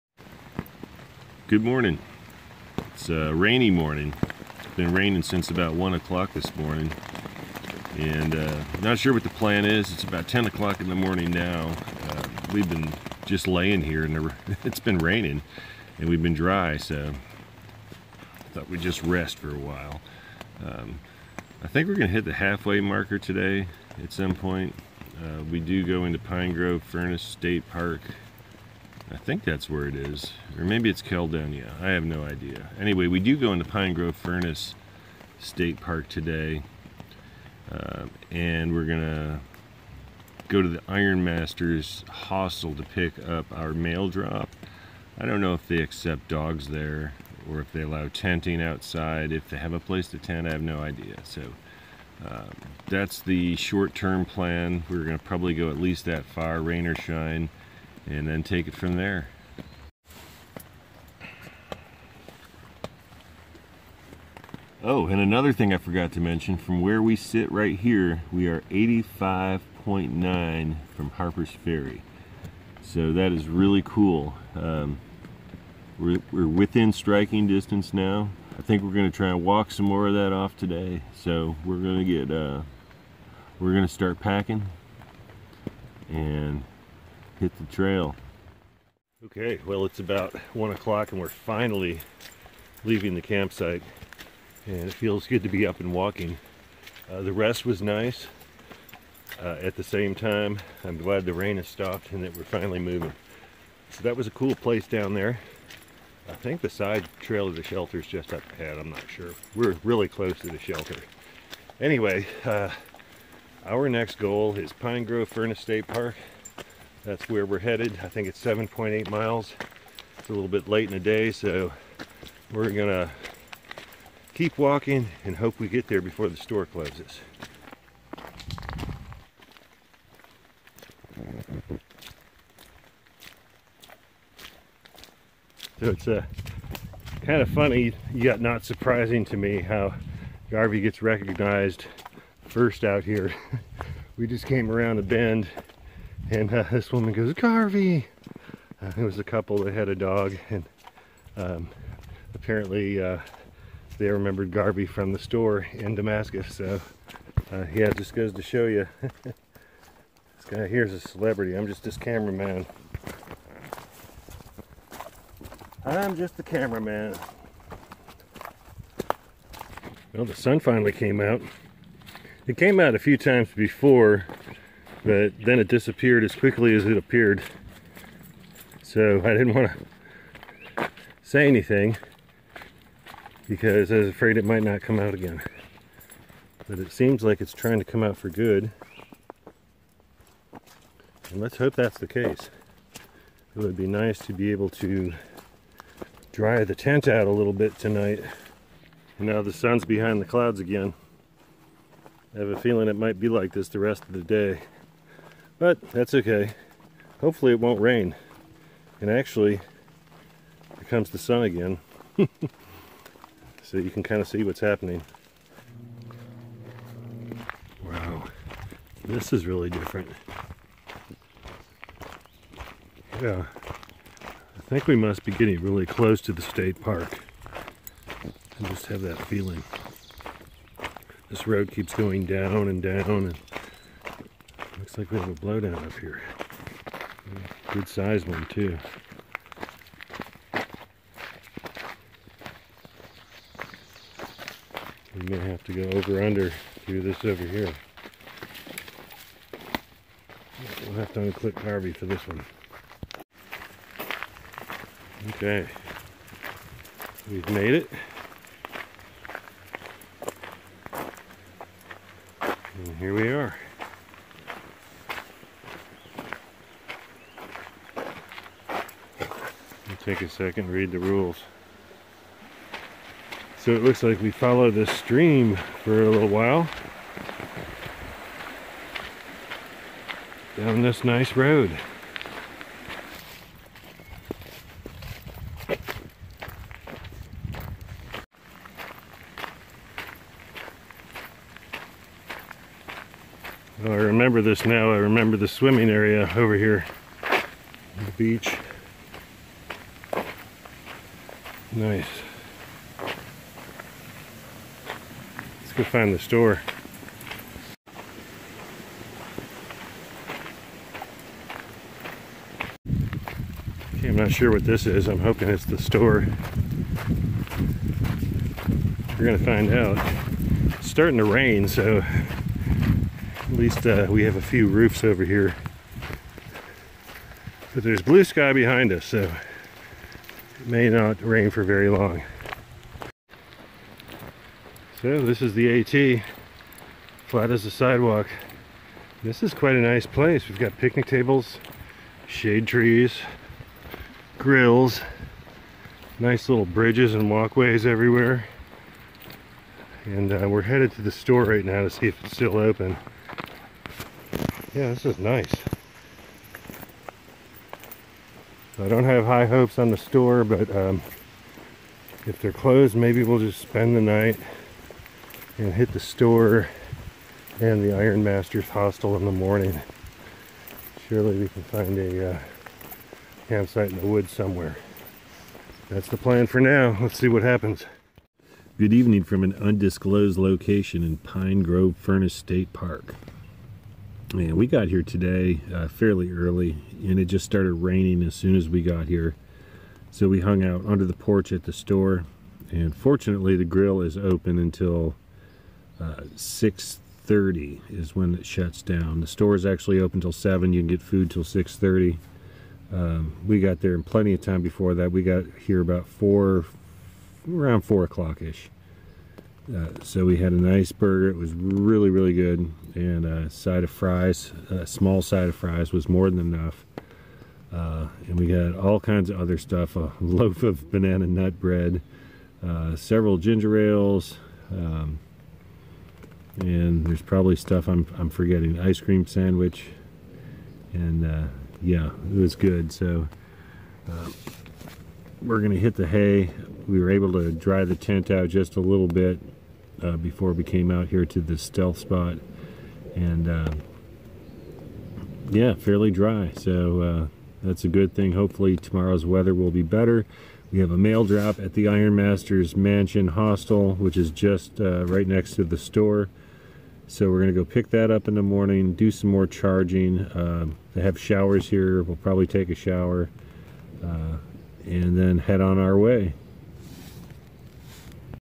GARVEY IS A STAR • PINE GROVE FURNACE STATE PARK
On day 24 we wake to rain and sleep- in before breaking camp.

Todays miles: 6.8
Total miles: 191.2